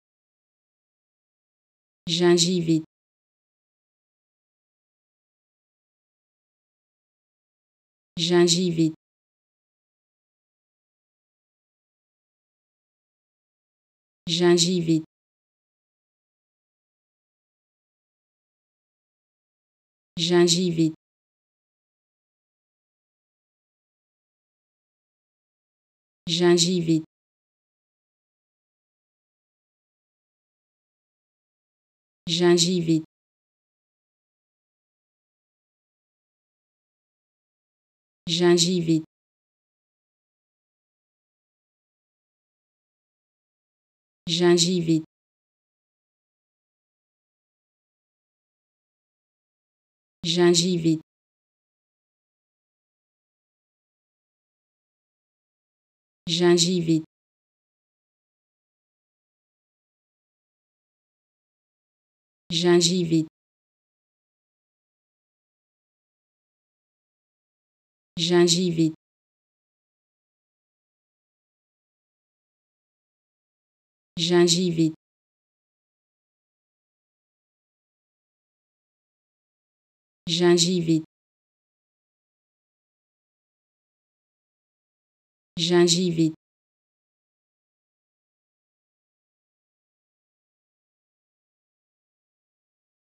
How to Pronounce ''Gingivite'' (Gingivitis) Correctly in French
Learn how to say and properly pronounce ''Gingivite'' in French with this free pronunciation tutorial.